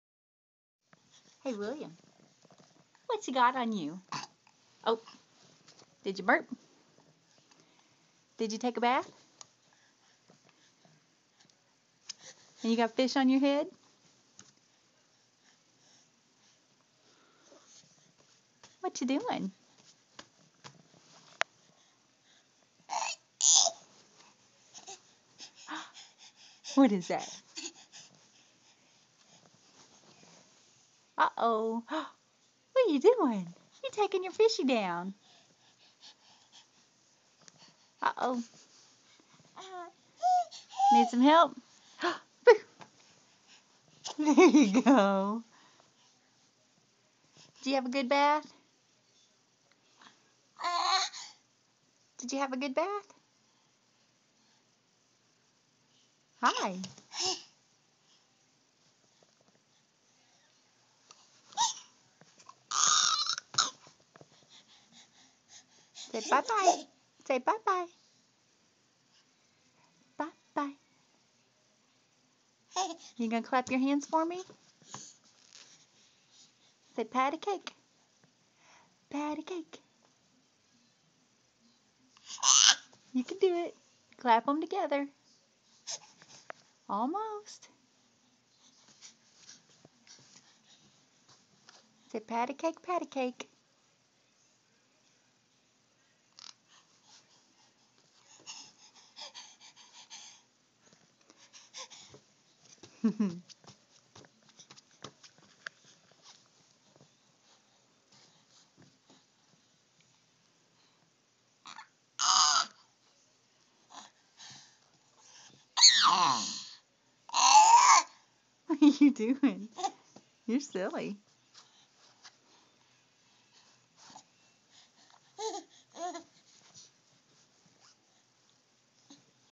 Bathtime baby
William just after his bath